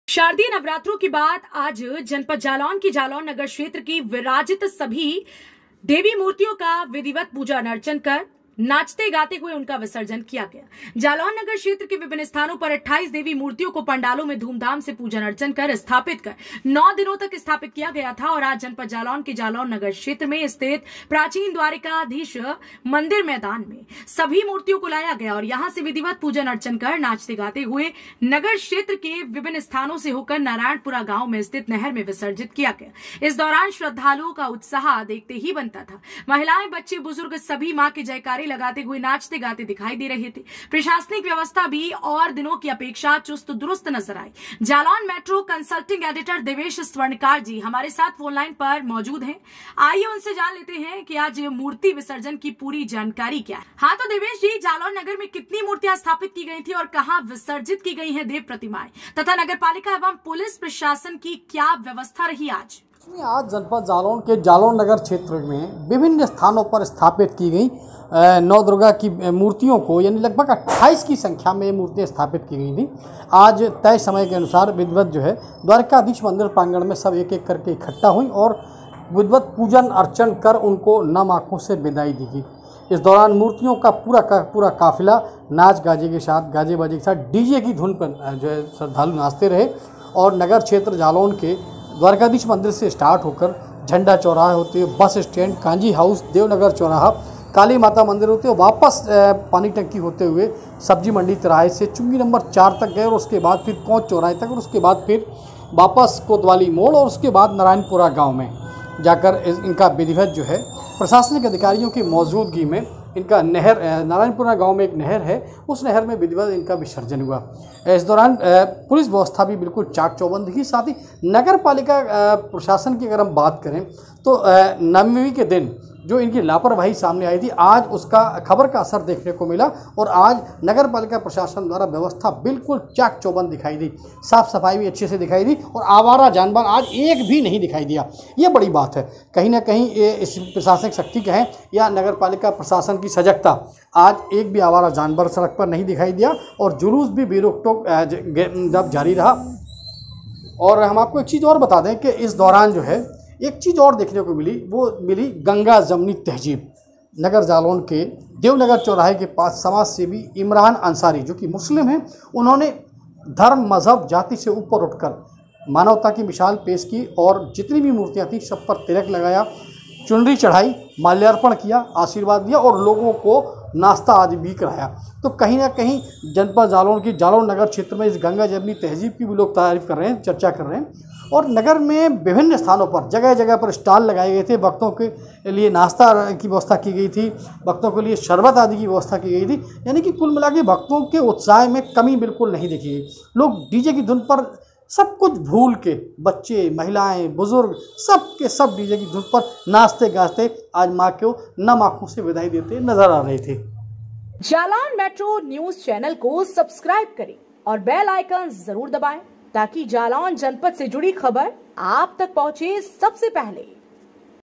जालौन नगर में विराजित सभी देवी मूर्तियों का विधिवत पूजन कर नाचते गाते हुए उनका विसर्जन किया गया
जालौन

रिपोर्ट - देवेश कुमार स्वर्णकार

मूर्तियों की विदाई

शारदीय नवरात्रों के बाद आज जनपद जालौन के जालौन नगर क्षेत्र की विराजित सभी देवी मूर्तियों का विधिवत पूजन अर्चन कर नाचते गाते हुए उनका विसर्जन किया गया
जालौन नगर क्षेत्र के विभिन्न स्थानों पर 28 देवी मूर्तियों को पंडालों में धूम धाम से पूजन अर्चन कर स्थापित कर नौ दिनों तक स्थापित किया गया था और आज़ जनपद जालौन के जालौन नगर क्षेत्र में स्थित प्राचीन द्वारिका धीष मंदिर मैदान में सभी मूर्तियों को लाया गया और यहां से विधिवत पूजन अर्चन कर नाचते हुए नगर क्षेत्र के विभिन्न स्थानों से होकर नारायणपुरा गांव में स्थित नहर में विसर्जित किया गया
इस दौरान श्रद्धालुओं का उत्साह देखते ही बनता था
महिलाएं, बच्चे, बुजुर्ग सभी मां के जयकारे लगाते हुए नाचते गाते दिखाई दे रहे थे
प्रशासनिक व्यवस्था भी और दिनों की अपेक्षा चुस्त दुरुस्त नजर आई